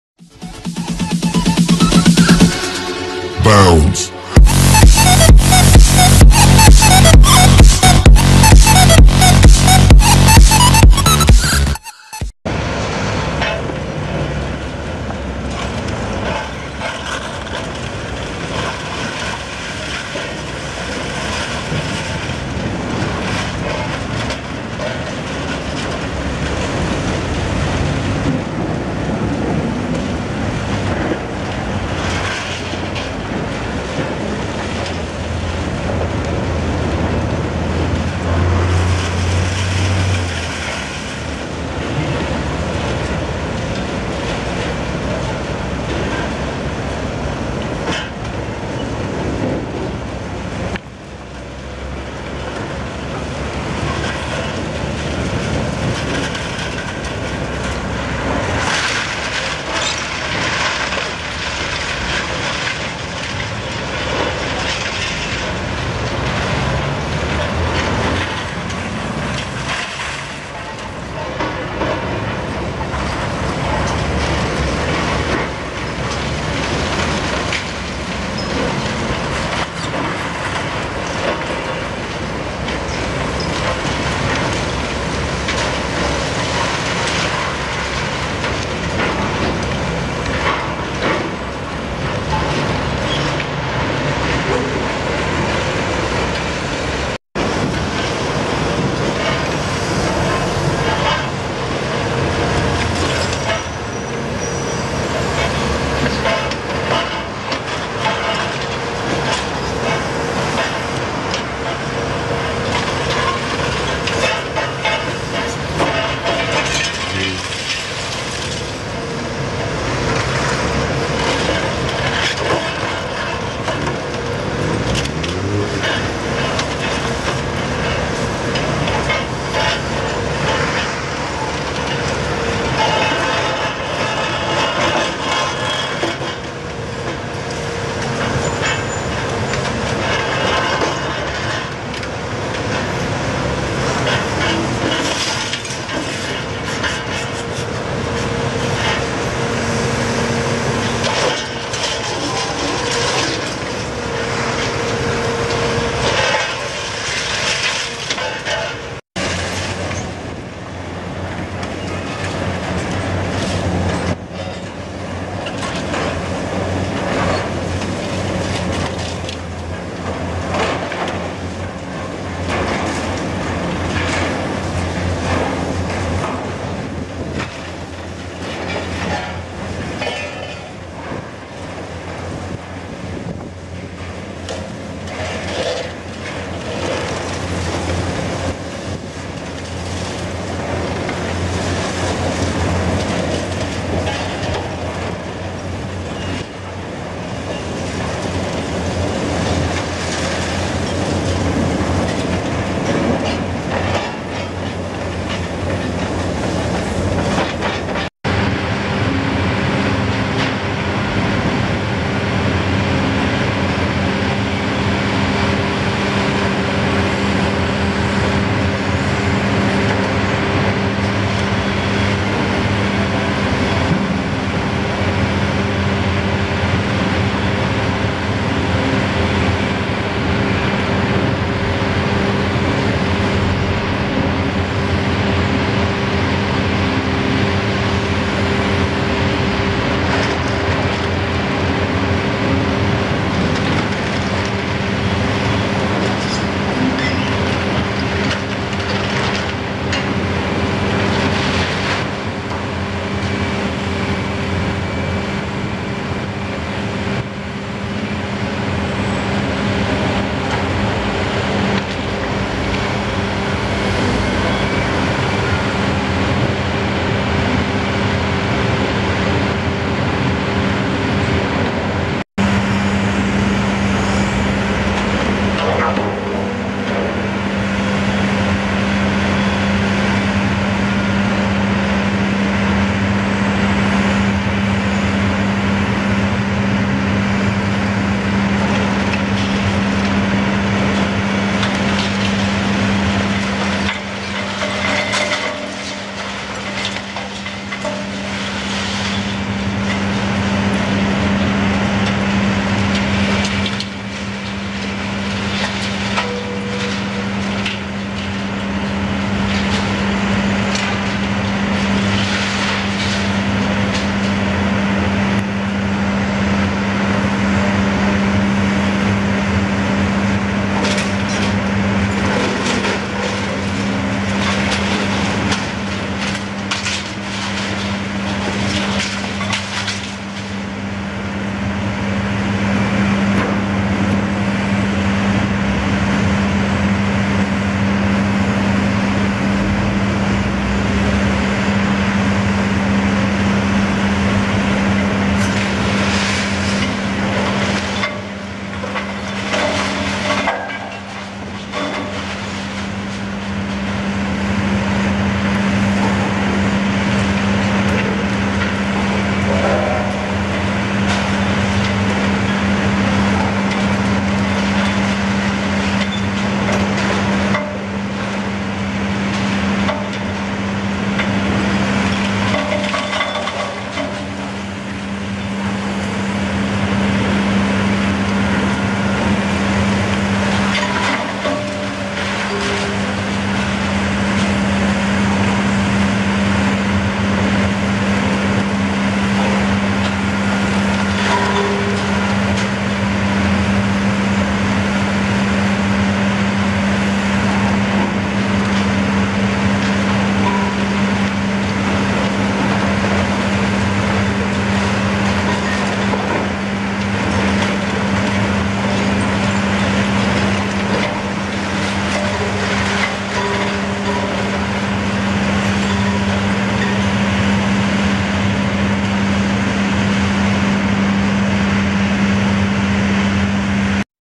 Excavator works at the construction site in Germany | Excavator most modern | excavator Mr.Coog
Composite video excavators, dump trucks, cranes, trucks in construction, roadworks, leveling